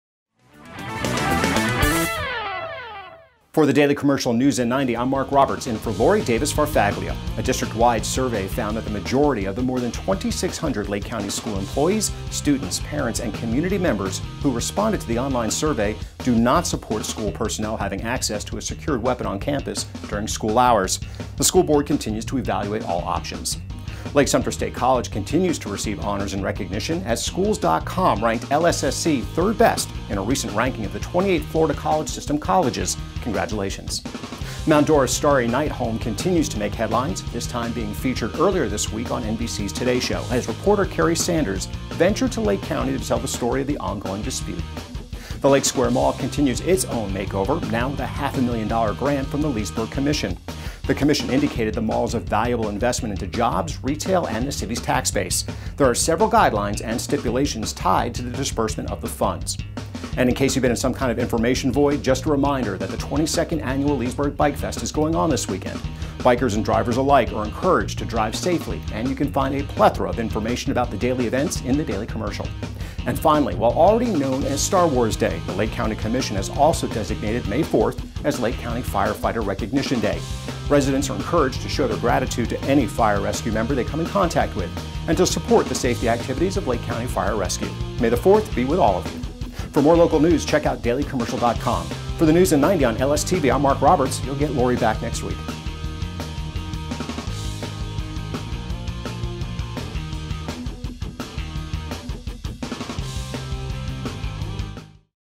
Daily Commercial's 'News In 90' April 19, 2018
Survey Shows Opposition To Arming School Staff
LSSC Ranks Third Best In State
Starry Night Home Featured On NBC's "Today Show"
Leesburg Approves $500K Grant For Mall
22nd Annual Leesburg Bikefest This weekend
May 4th, Firefighters' Recognition Day